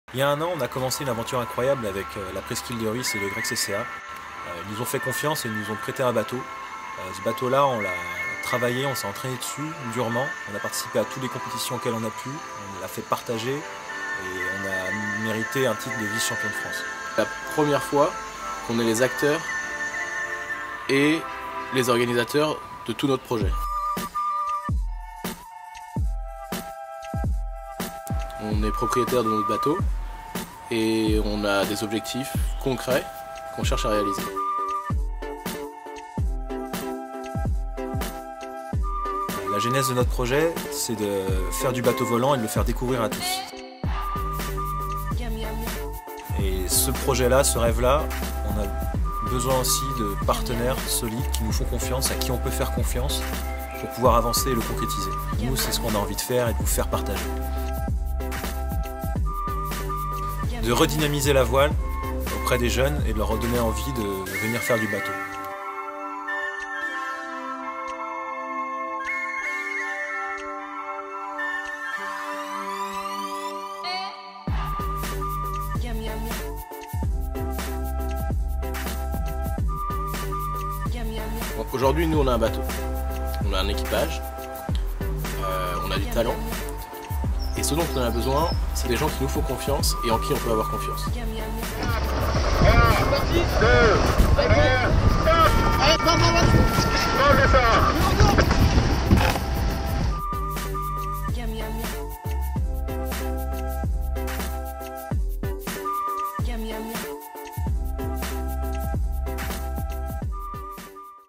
Projet Moth De Rhuys 2015-2017
Equipe de voile sportive YCCA/SRV de la presqu'ile de Rhuys en préparation pour le Tour de France à la voile 2017 et le circuit MOTH/FLYING